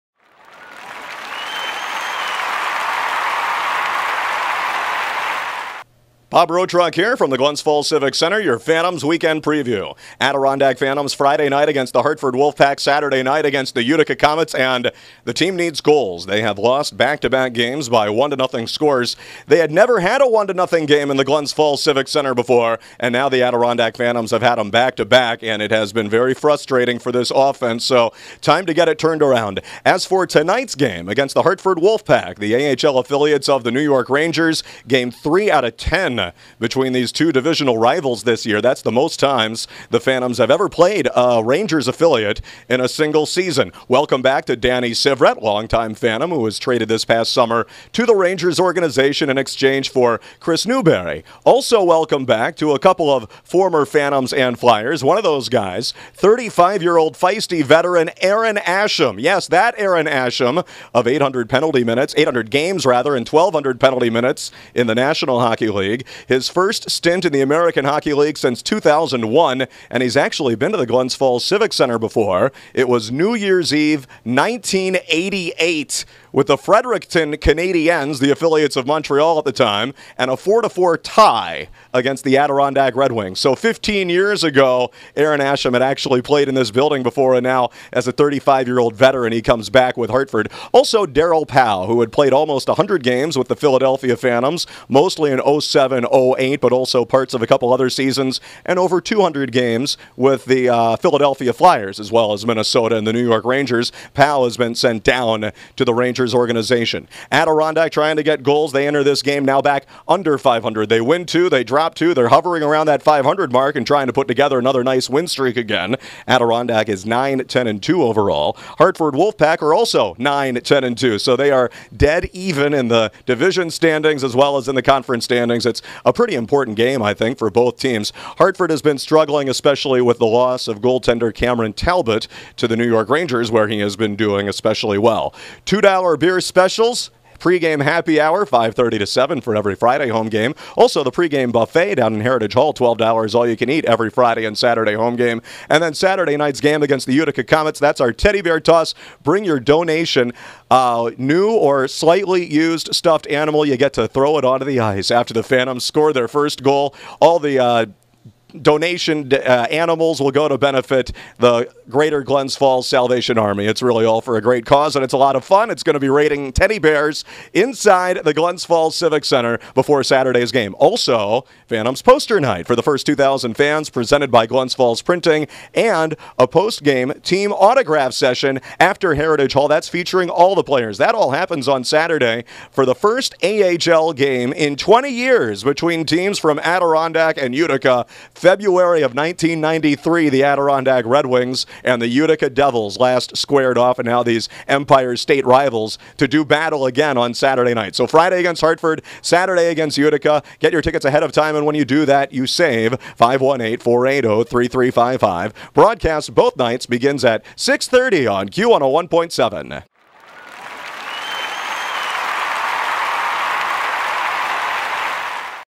PhantomsTV! Weekend Preview Dec 6-7
Bob Rotruck checks in from the Glens Falls Civic Center looking ahead to this weekend's games against Hartford and Utica.